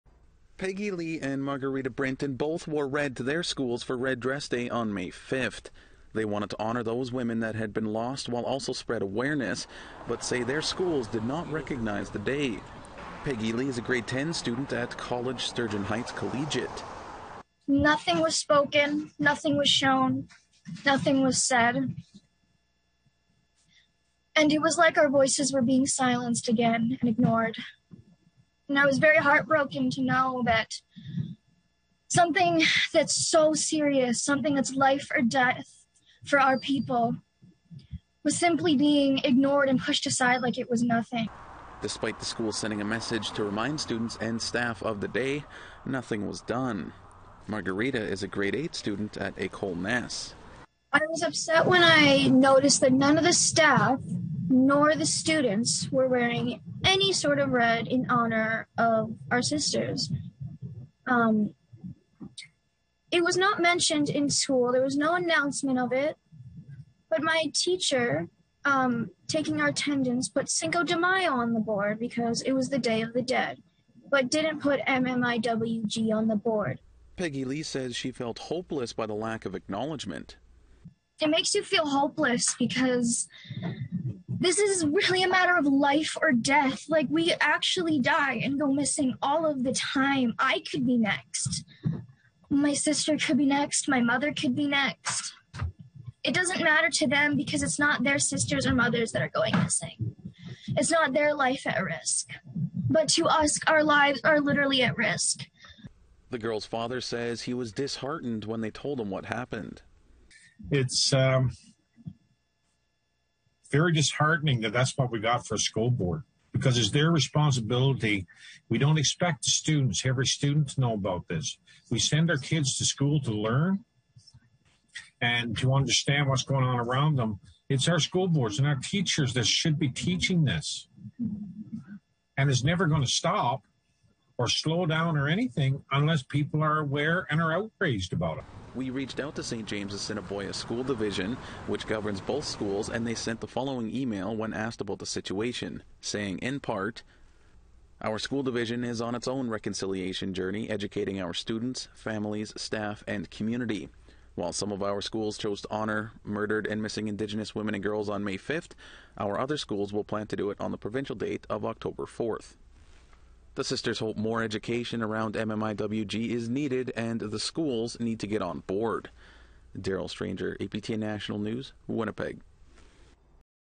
Two students disappointed their schools didn’t honour Red Dress Day
Many honoured Red Dress Day last week, including a pair of Winnipeg sisters who wanted to show their support.

But they say they were disappointed their schools didn't honour the day.